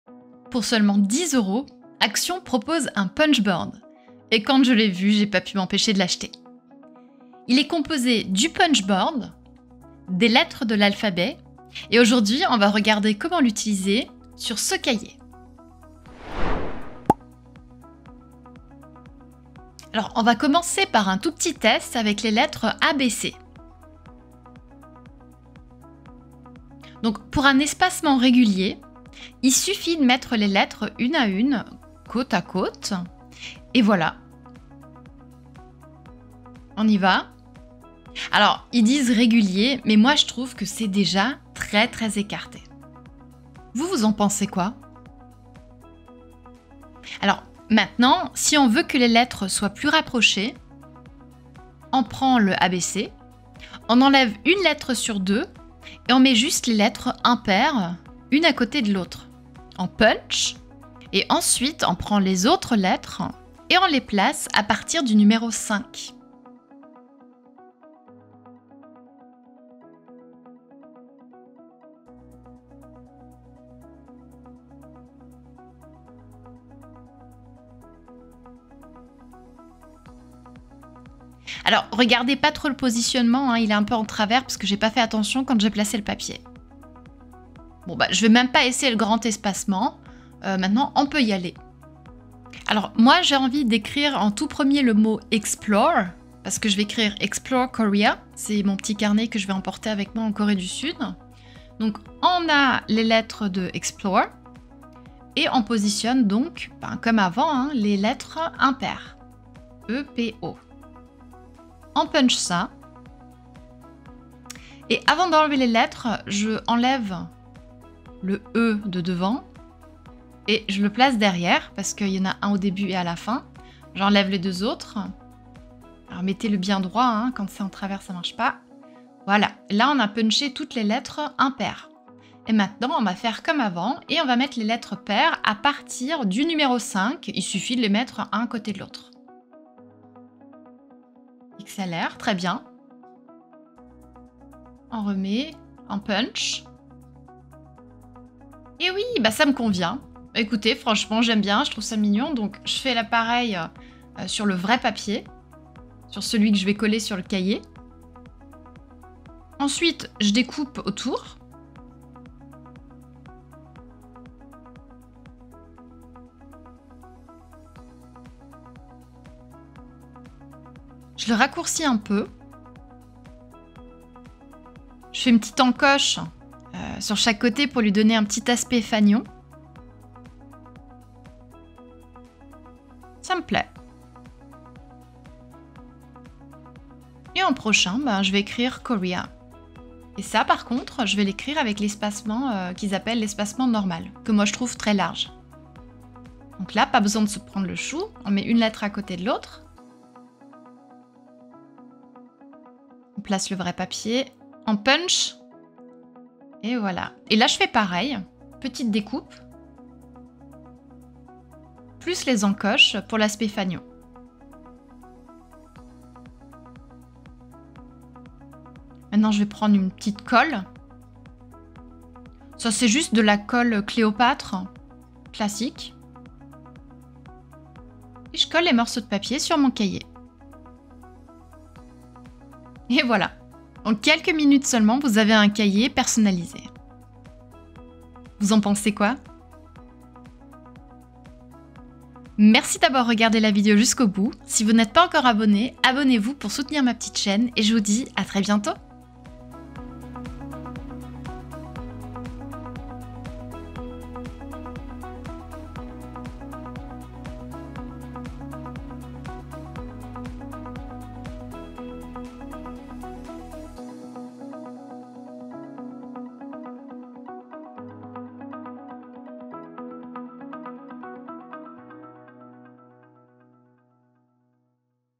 DIY Rapide avec le Punch Board d'ACTION
Dans cette vidéo, je vous montre comment utiliser un punch board, un outil fantastique pour les amateurs de scrapbooking et de DIY. Après avoir déniché ce gadget chez ACTION pour seulement 10 €, j'ai décidé de le mettre à l'épreuve en personnalisant un carnet que j’emmènerai lors de mon prochain voyage en Corée. Suivez-moi étape par étape pour apprendre à aligner et espacer parfaitement les lettres pour créer un titre accrocheur sur votre carnet. Que vous soyez débutant ou expérimenté en matière de DIY, vous trouverez des astuces utiles pour rendre vos projets uniques.